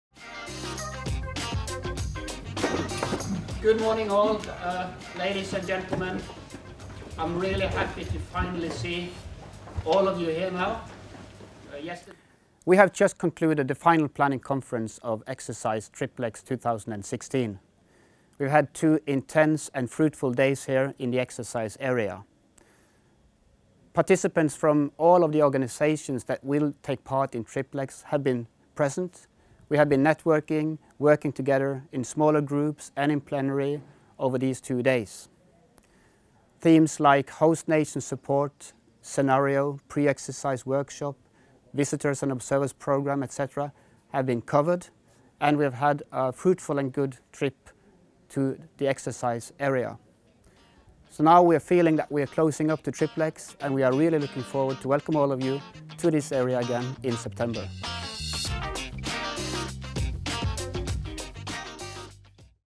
FPC Triplex 2016
Geir Ellingsen, project manager for Triplex 2016 gives us some glimpses from the FPC (Final Planning Conference) held in May 2016. IHP-Triplex 2016 is a full-scale, civilian-led, humanitarian field exercise, with the aim to practice the international humanitarian response. Sept. 25-30 in Norway.